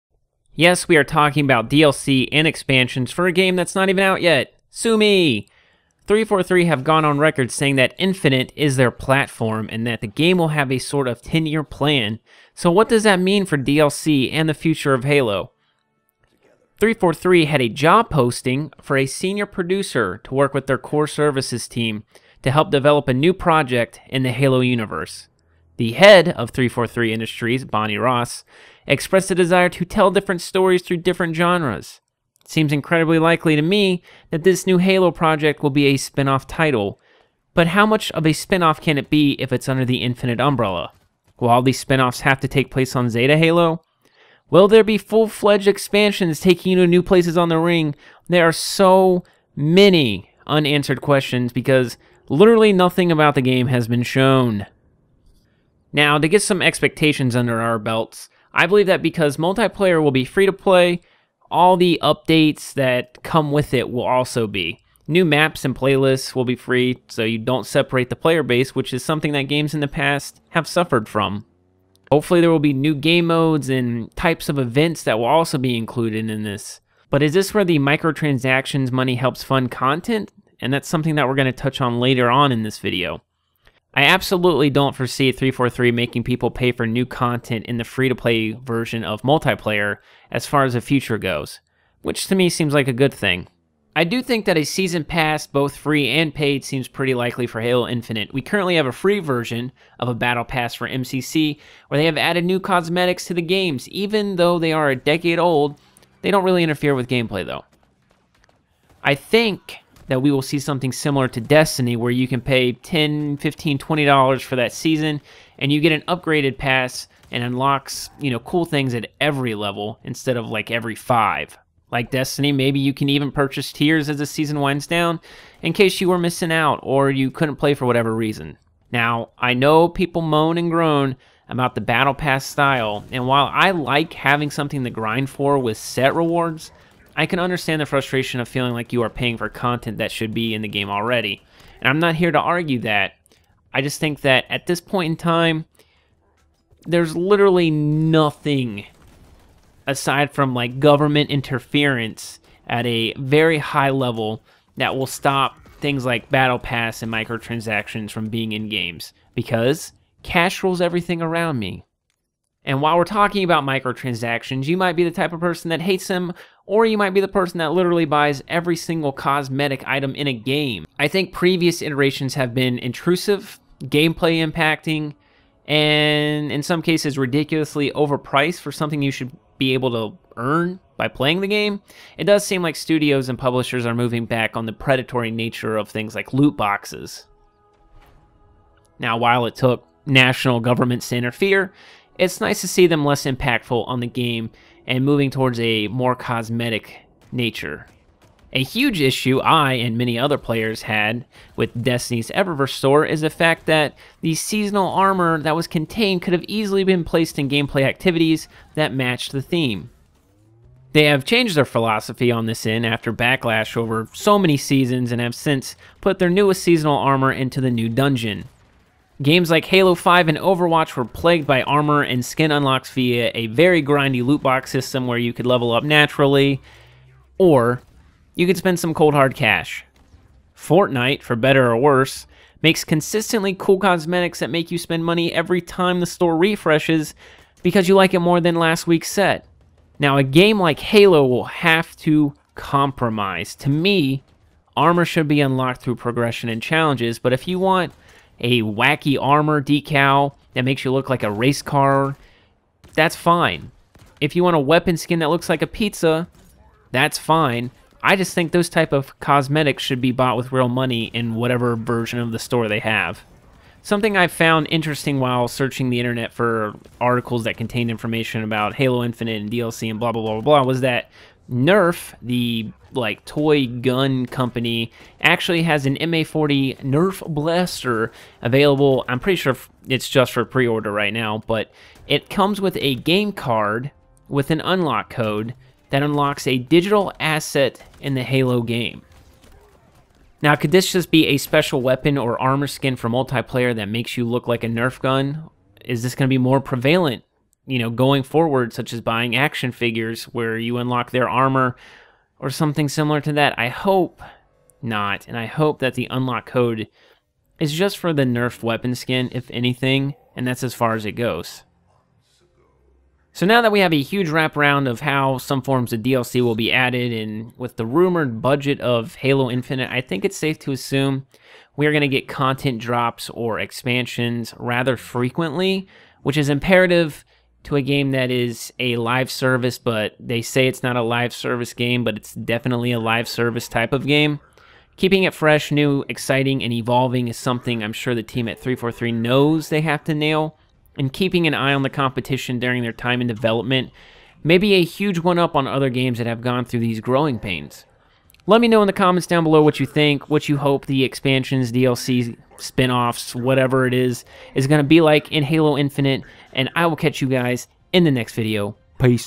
Halo Infinite DLC, Expansions, and Microtransactions
Going over how i see 343 handling the micro transactions and how DLCs and expansions will pan out in Halo Infinite.